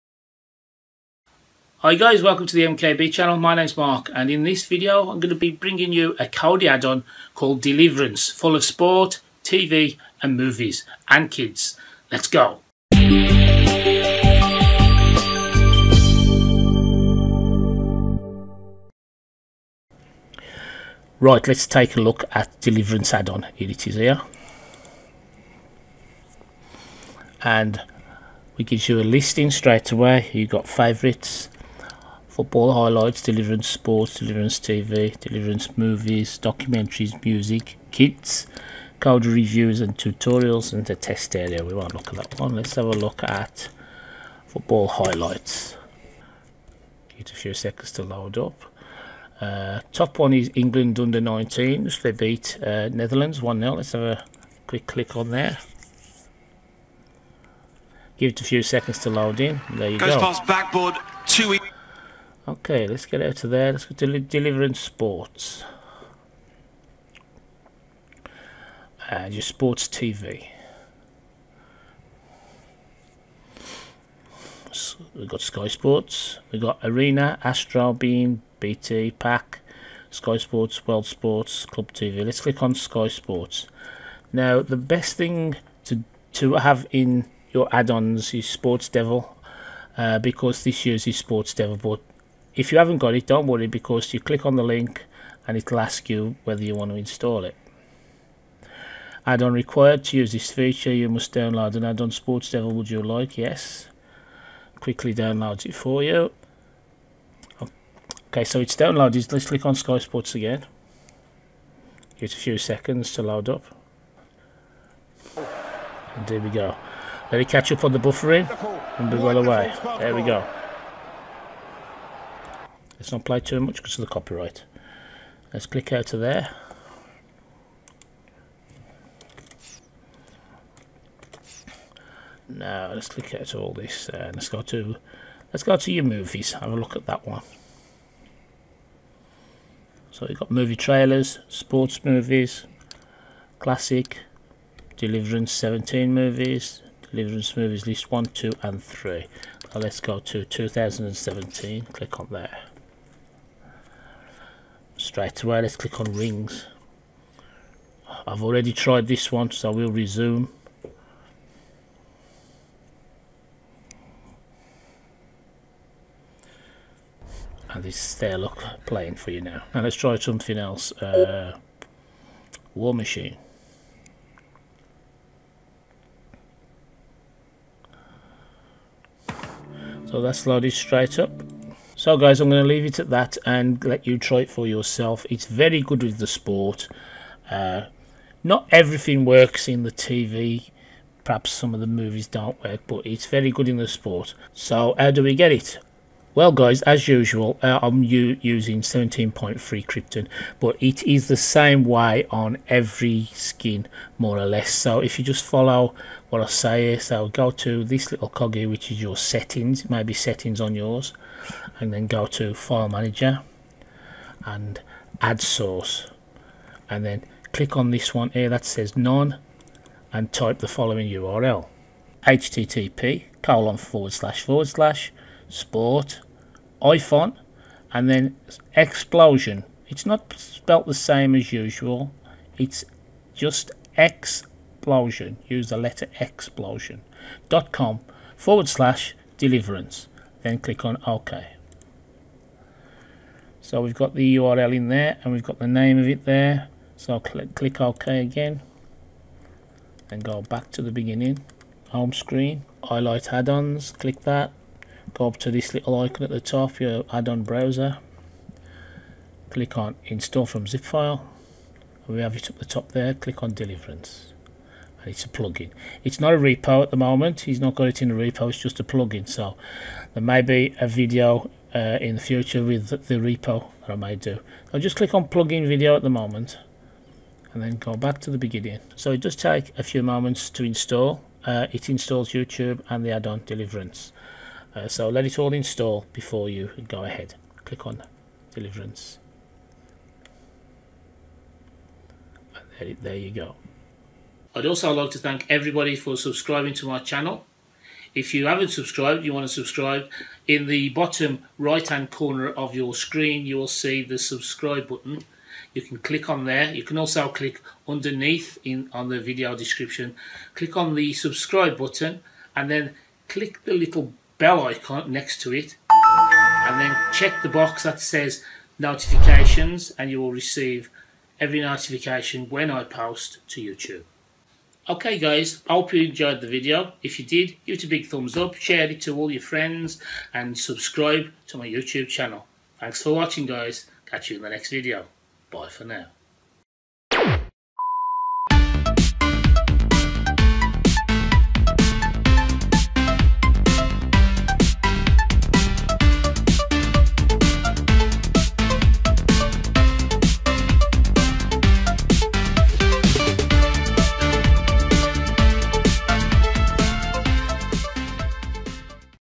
Deliverance Best add-on for sports, TV, Movies and Kids
Sport, Movies, TV Shows and Kids content all in this 3rd party add-on for Kodi